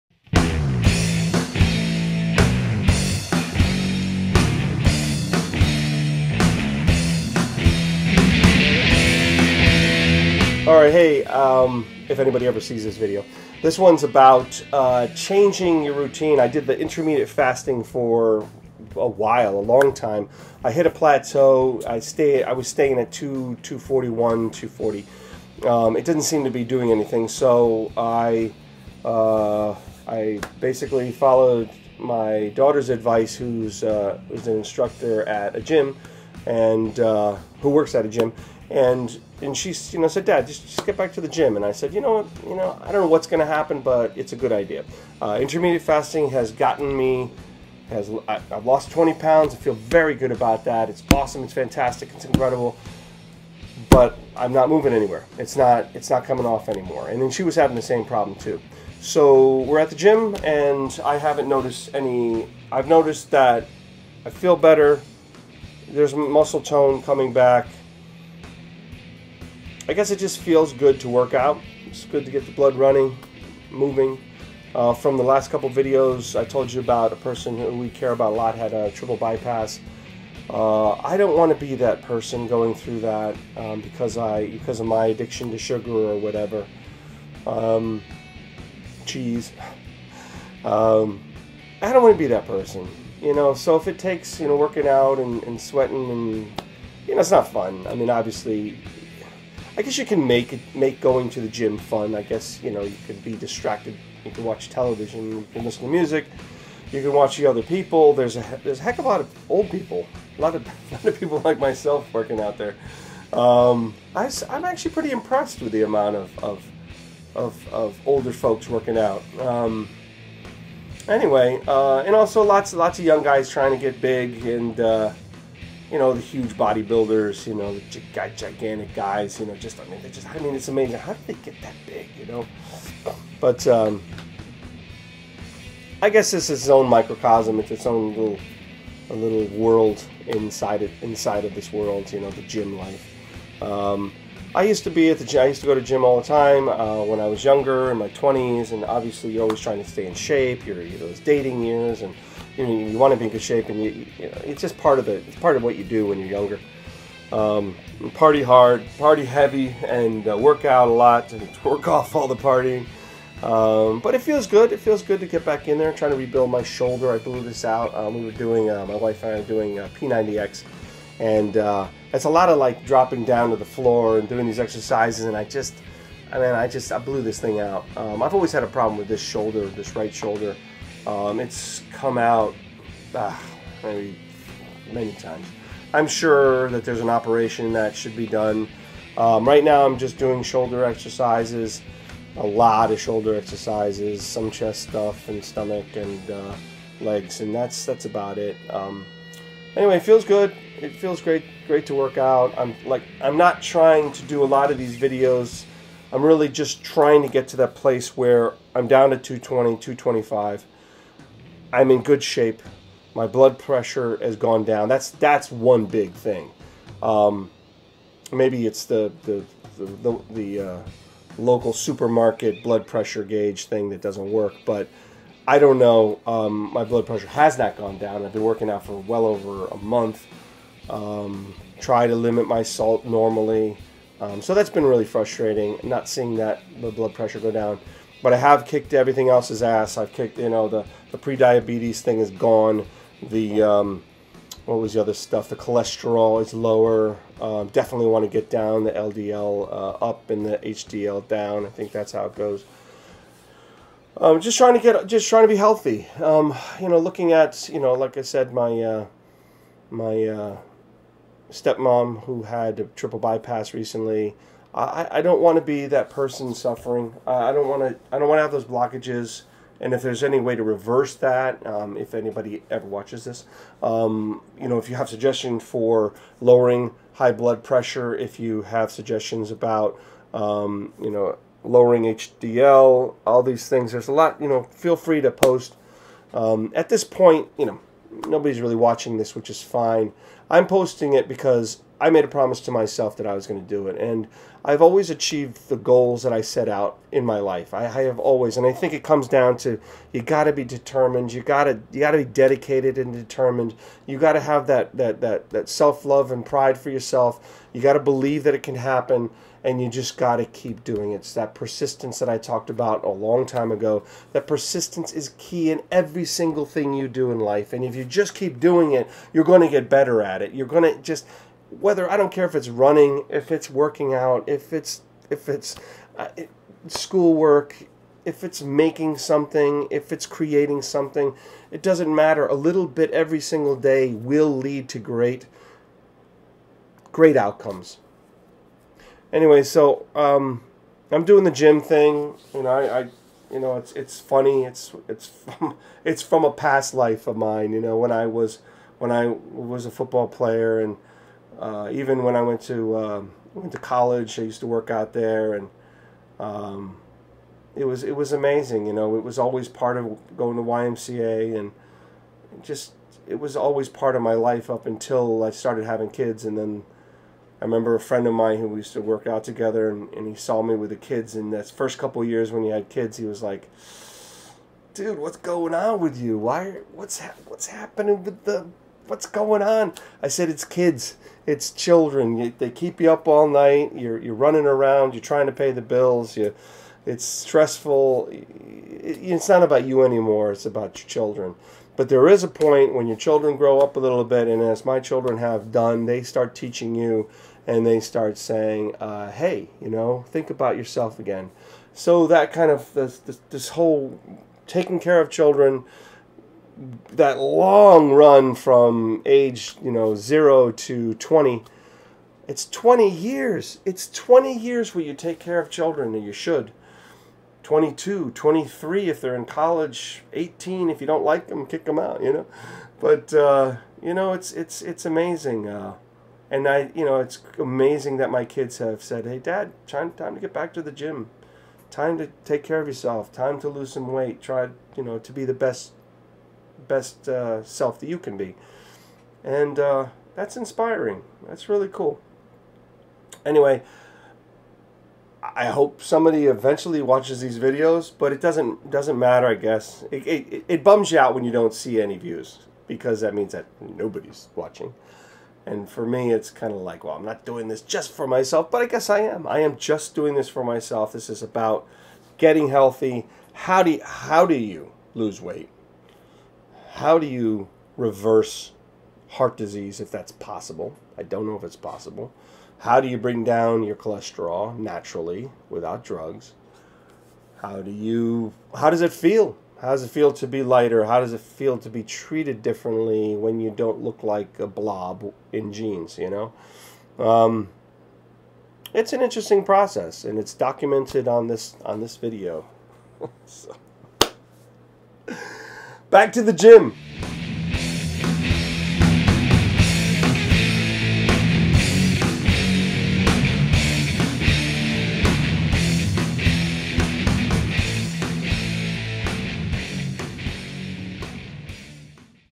Day Twenty Seven "Back to the gym"
After stagnating at 240, I decided to do something different and go to the gym. Blood pressure needs to come down and I was in desperate need of real physical exercise, so it's gym time!!